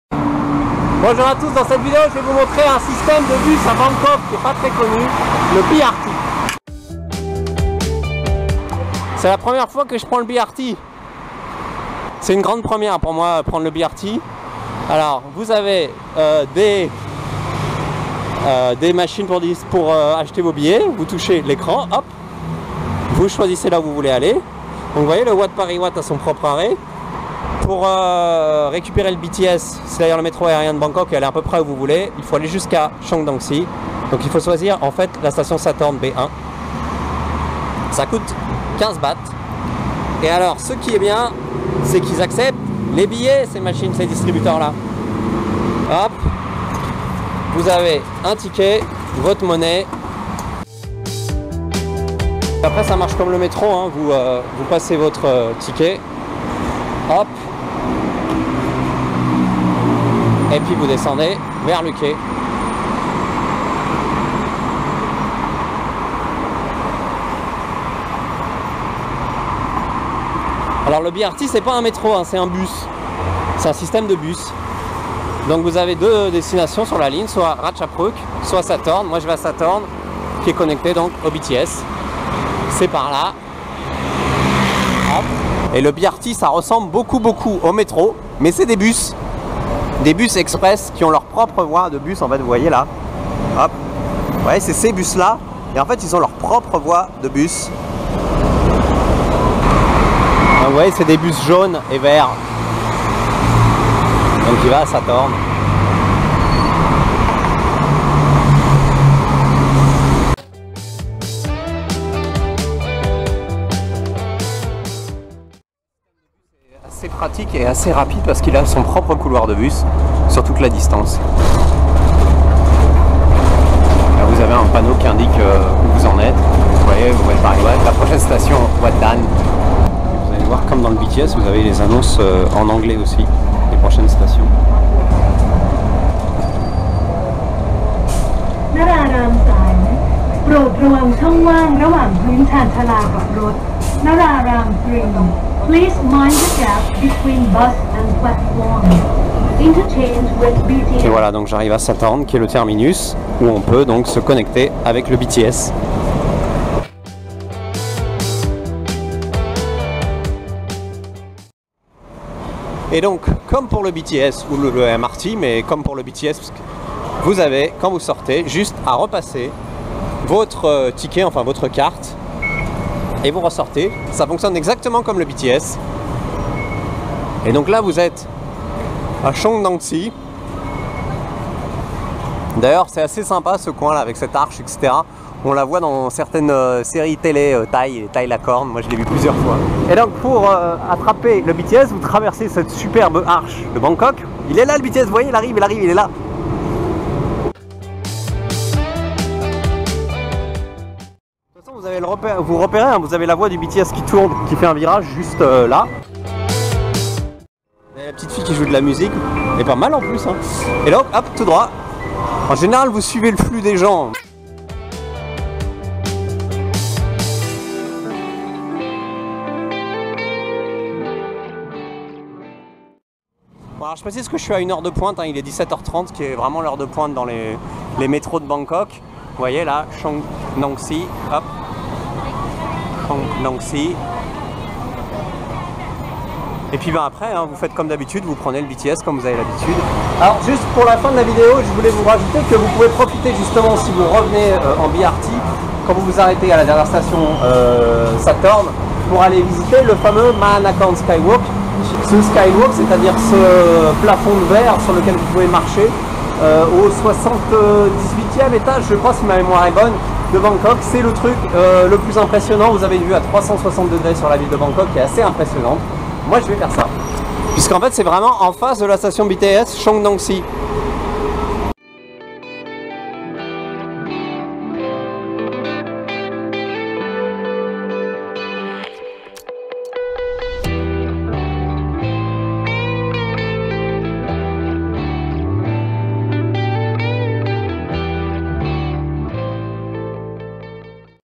Se déplacer à Bangkok: les BRT bus
Petite vidéo de présentation des BRT bus à Bangkok, utiles notamment pour aller à l'étonnant Wat Pariwat et ses personnages de mangas et Marvel disséminés dans les décorations extérieures du temple.